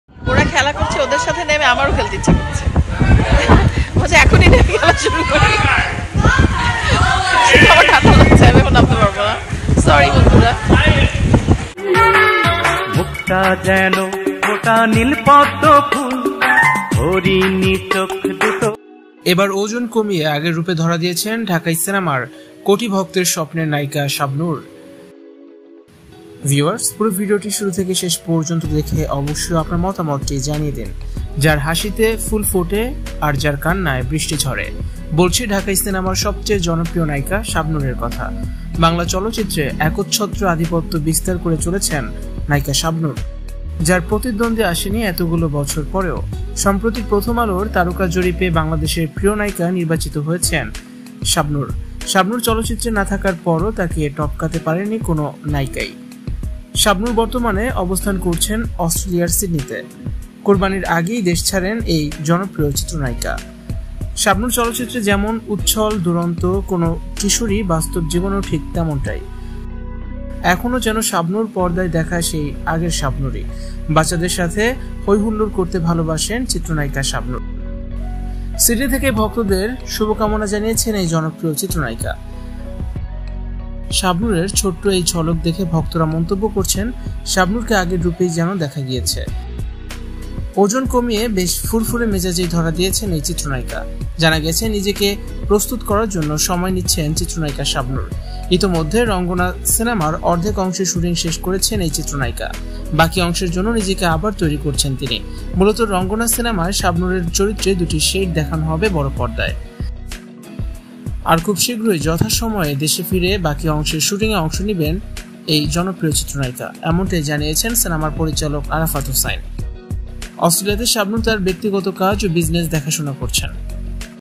দেখুন ওজন কমিয়ে আগের রূপে শাবনূর চমক দিলেন!কি বলছে শাবনূর!
দেখুন ওজন কমিয়ে আগের শাবনূর চমক দিলেন!কি বলছে শাবনূর!